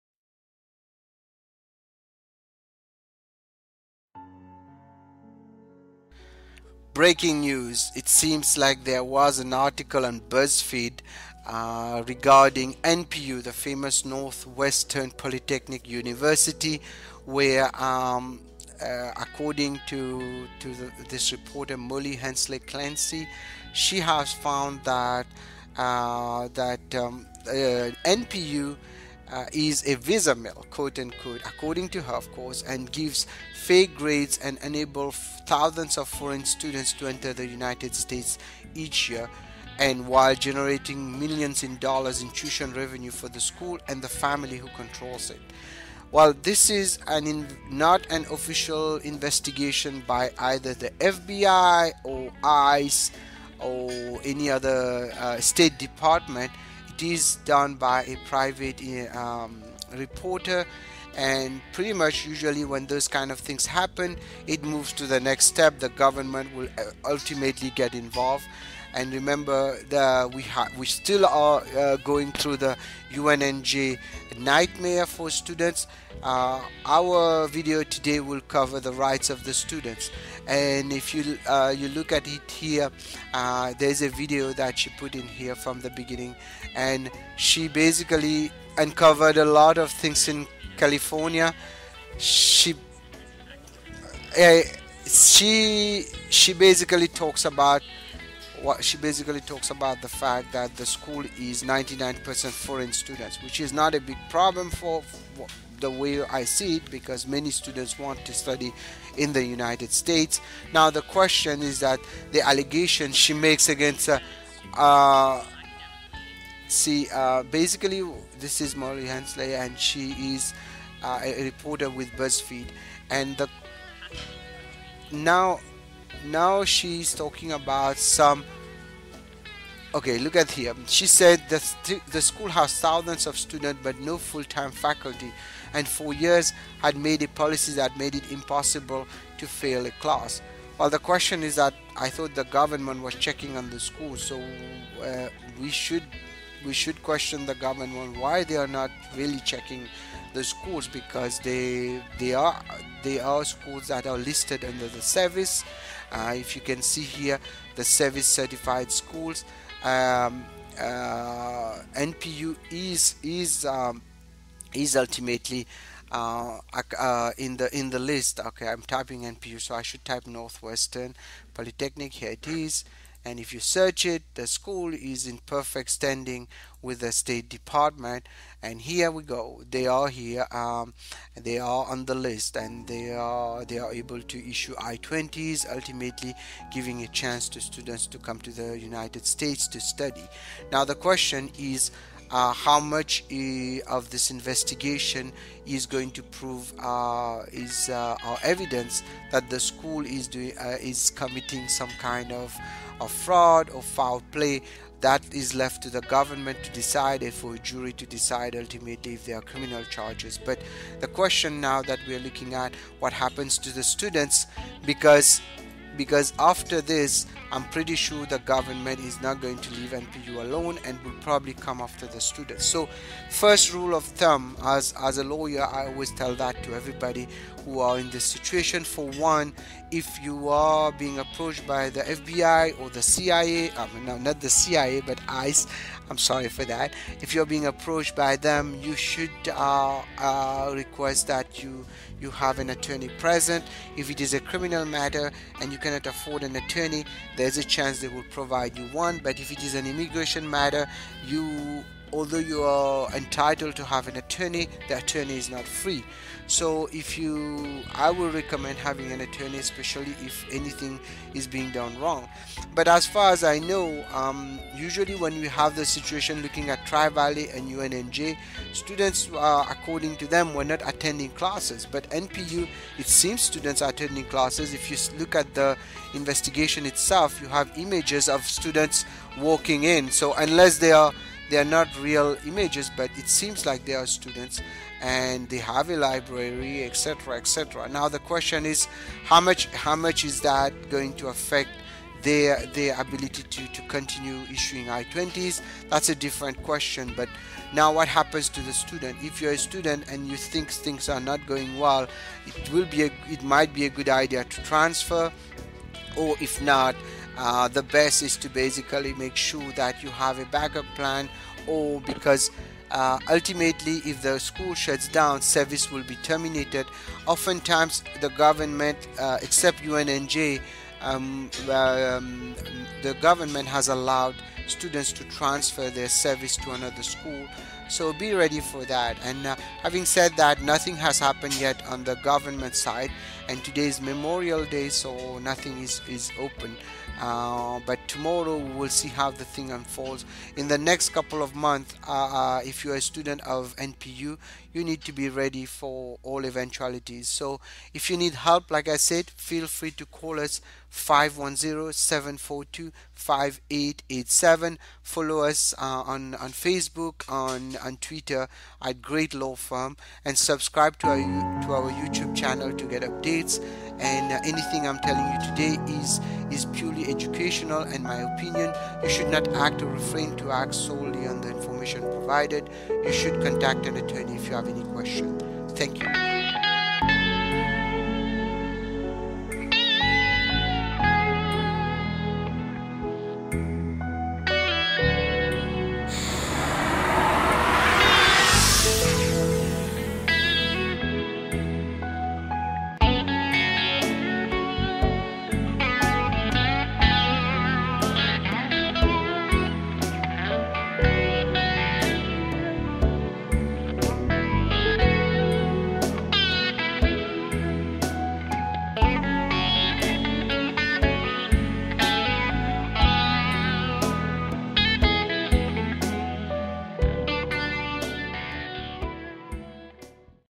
Breaking News: Is NPU next? Foreign Students - Know Your Rights!
Is NPU (NorthWestern Polytechnic University) next in the list of investigated University?

An article on Buzzfeed on May 26 2016 describes NPU as “A college on the edge of Silicon Valley has turned itself into an upmarket visa mill, a BuzzFeed News investigation has found, deploying a system of fake grades and enabling thousands of foreign students to enter the United States each year — while generating millions of dollars in tuition revenue for the school and the family who controls it. “

The video covers the rights of the students and to all NPU students please be ready for all eventualities.